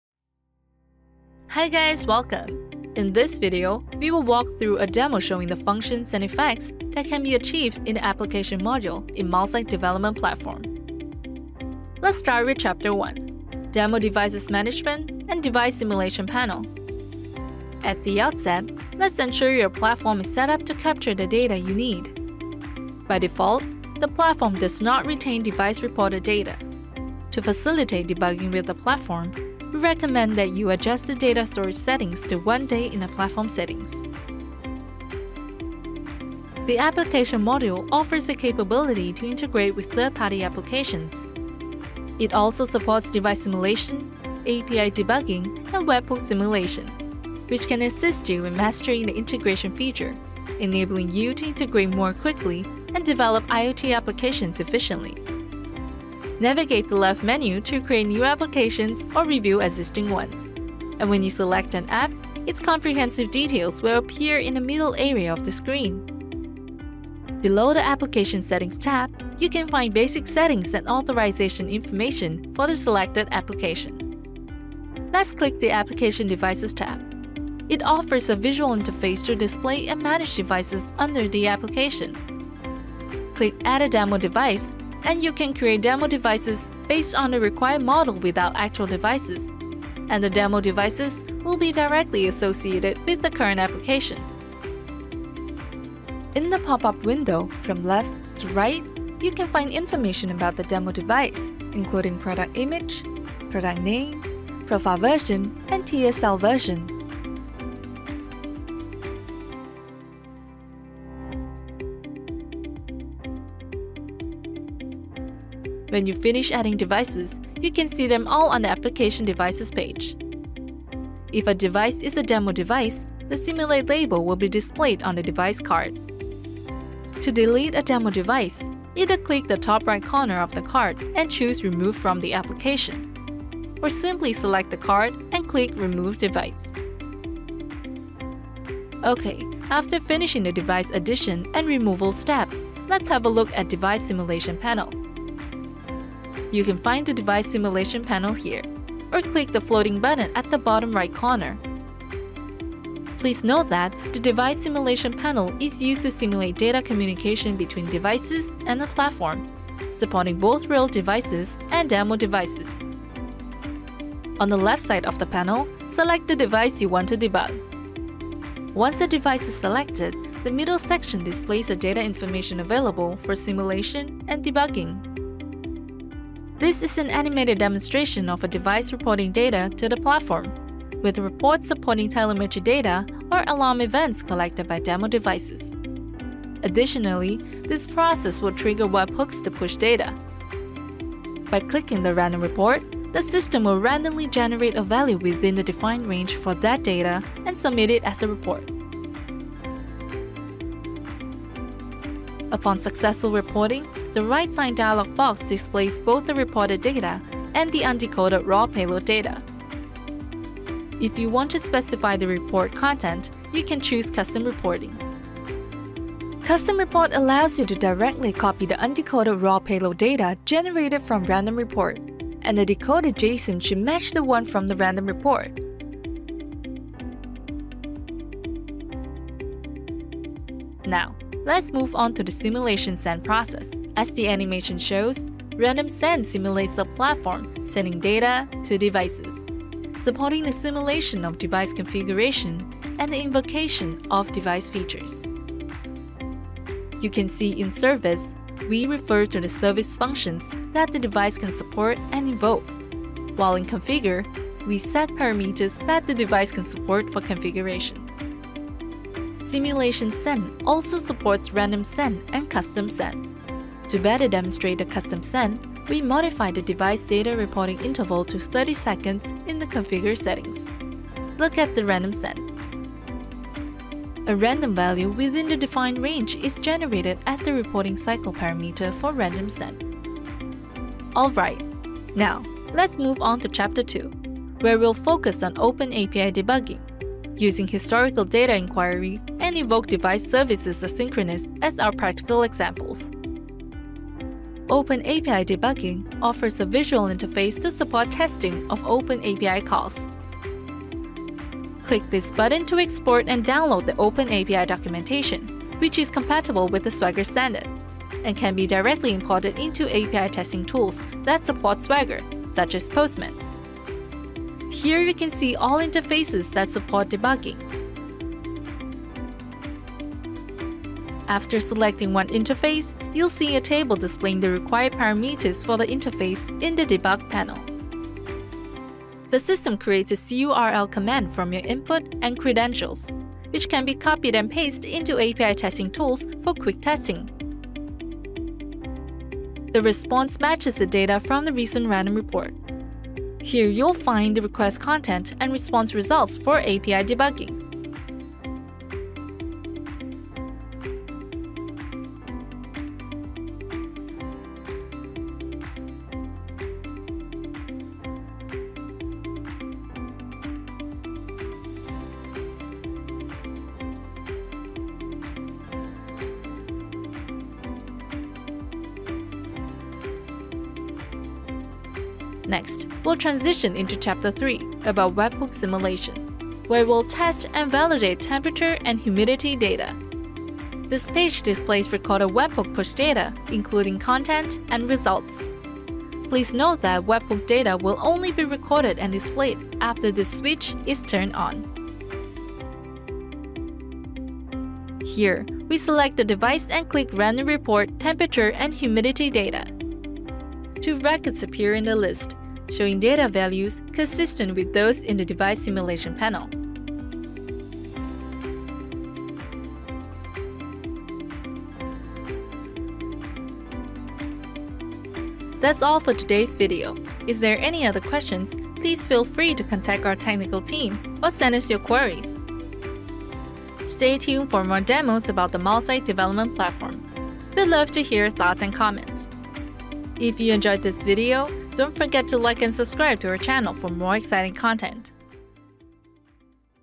Application & Device Debugging of Milesight Development Platform
Dive into the advanced features of the Milesight Development Platform with our latest demo video. Unleash the full potential of the Application Module:

1️⃣ Demo Devices Management & Simulation: Learn how to manage and simulate devices with ease, setting the stage for efficient development.
2️⃣ OpenAPI Debugging: Dive into API debugging to ensure your applications are robust and reliable.
3️⃣ Webhook Simulation: Test real-time responsiveness with our webhook simulation features.